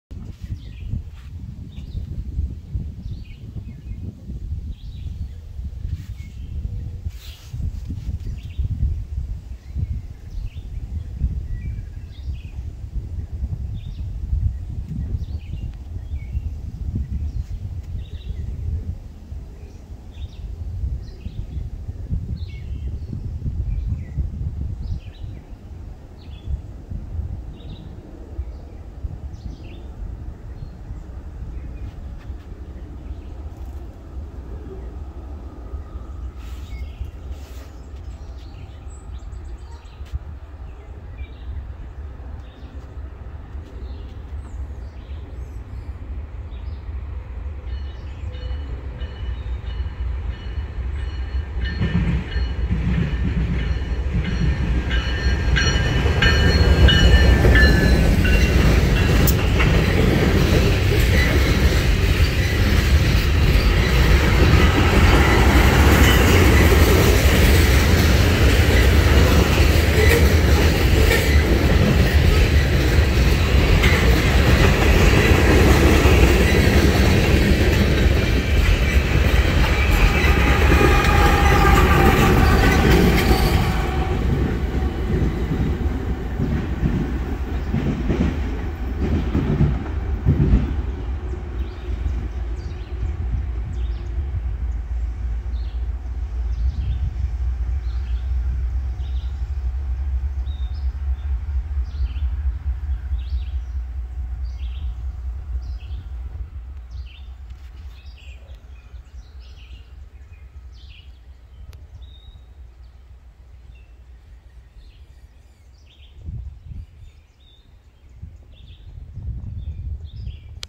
Norfolk Southern #7594 leads a very short 17 car manifest at Lewistown, PA (06/10/2023)
This is a 10G that dropped off a lot of cars in Altoona, and as such only has 17 cars. Short enough that I could get the entirety of it in a YouTube Short.